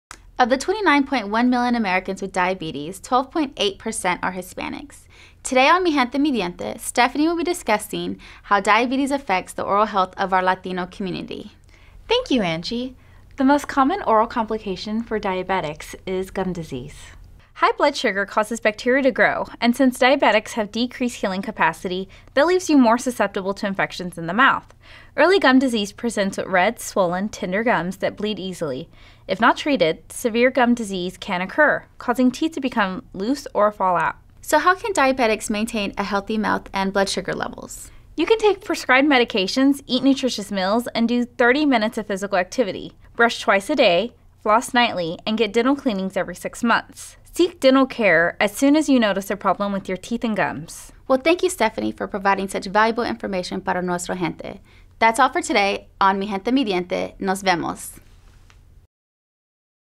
Mi Gente Mi Diente
"My People My Teeth" The Hispanic Student Dental Association from UT Health Science Center School of Dentistry in San Antonio, explains the most common oral complication found in Diabetic patients. HSDA provides information  on what to expect when gum disease presents itself and gives some tips on how to maintain a healthy mouth!